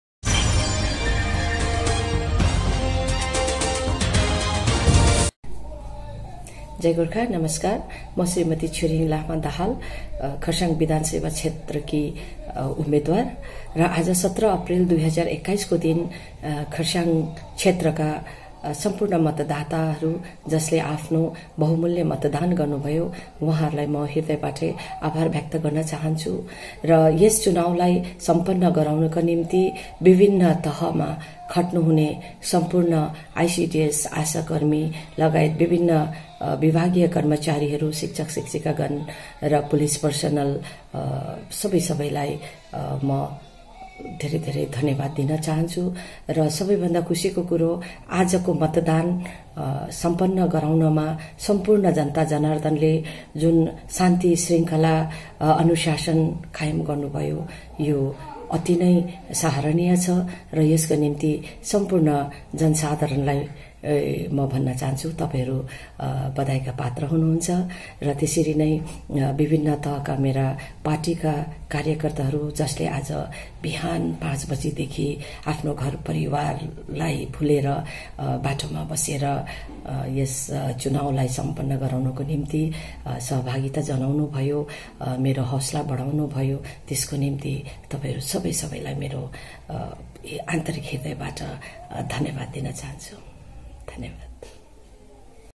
"आजको चुनाव शान्ति पूर्ण सम्पन्न भयो जुन सराहनीय कुरा हो। यसको निम्ति जनसाधारण बधाइका पात्र हुन्।
"आजको चुनाव शान्ति पूर्ण सम्पन्न भयो जुन सराहनीय कुरा हो। यसको निम्ति जनसाधारण बधाइका पात्र हुन्।"छिरिङ लामा दाहाल

खरसाङ विधान सभा क्षेत्रका सम्पूर्ण मतदाताहरु जसले आफ्नो बहुमूल्य मतदान गर्नु भयो मेरो हृदय बाट नै आभार प्रकट गर्न चाहन्छु। यस चुनाव लाई सम्पन्न गराउन विभिन्न तहमा खट्नु  हुने सम्पूर्ण कर्मचारीहरु आई सी डी एस, आशा कर्मचारी, शिक्षक शिक्षिका, विभिन्न विभागका कर्मचारी , पुलिस सबैलाई धेरै धेरै धन्यवाद दिन चाहान्छु।

सबै भन्दा खुशीको कुरा आजको चुनाव शान्ति पूर्ण सम्पन्न भयो जुन सराहनीय कुरा हो। यसको निम्ति जनसाधारण बधाइका पात्र हुन्।

मेरो पार्टीका सबै कार्यकर्ताहरु जसले आज बिहान देखि नै चुनावलाई सम्पन्न गराउन अनि मेरो हौसला बढाउन सहभागी बन्नु भयो सबैलाई मेरो आन्तरिक हृदयबाट नै धन्यवाद।

छिरिङ लामा दाहाल